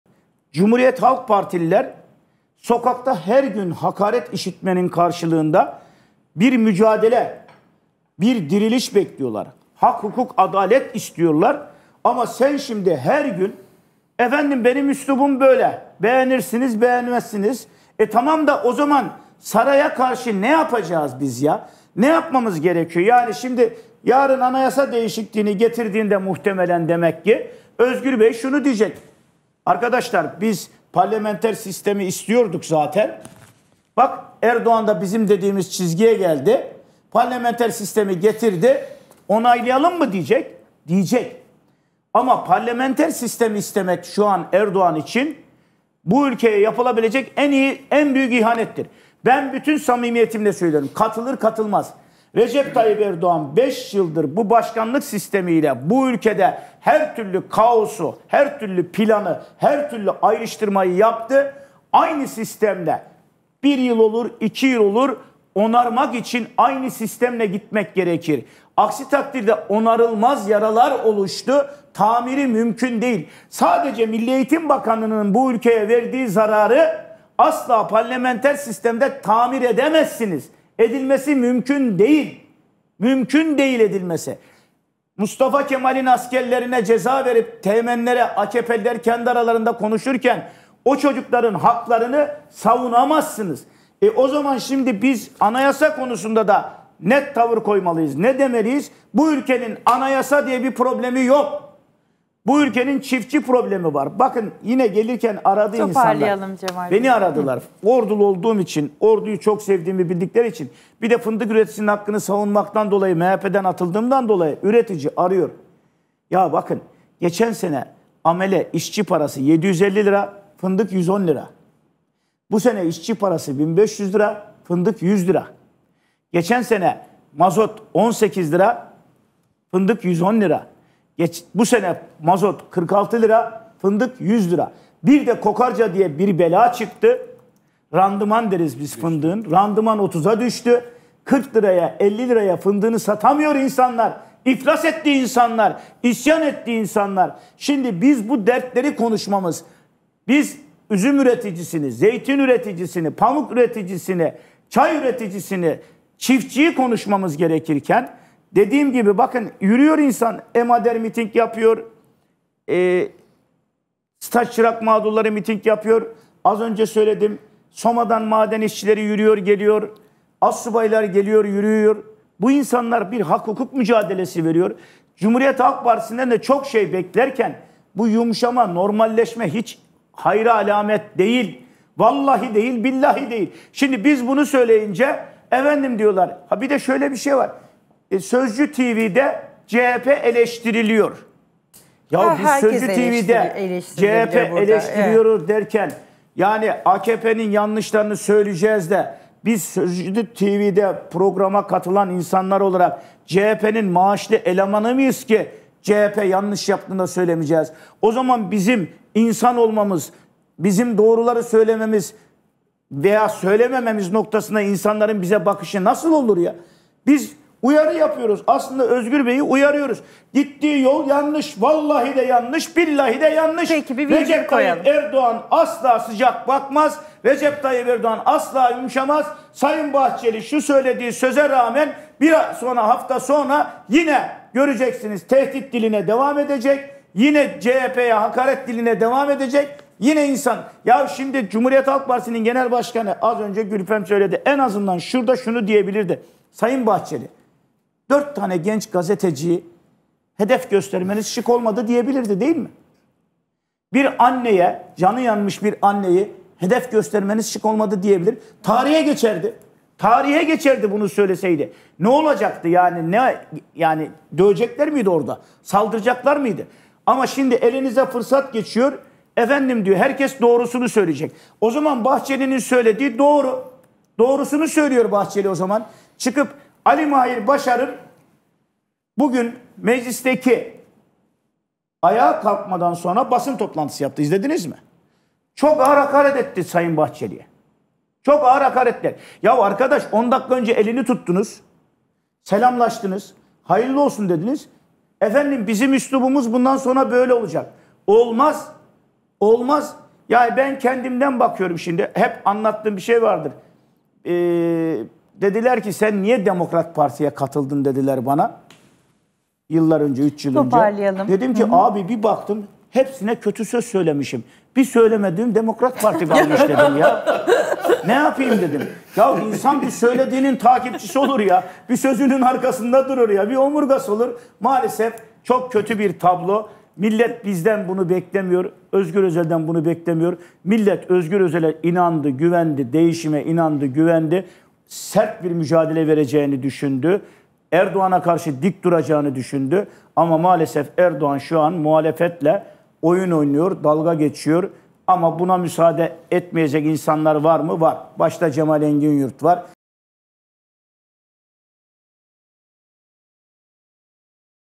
Erdoğan Parlamenter Sisteme Dönebilir! Cemal Enginyurt'tan Tarihi Anayasa Cevabı
Digiturk: 60
D-smart: 93
Turkcell TV Plus: 69
Tivibu: 69
Türksat Kablo TV: 76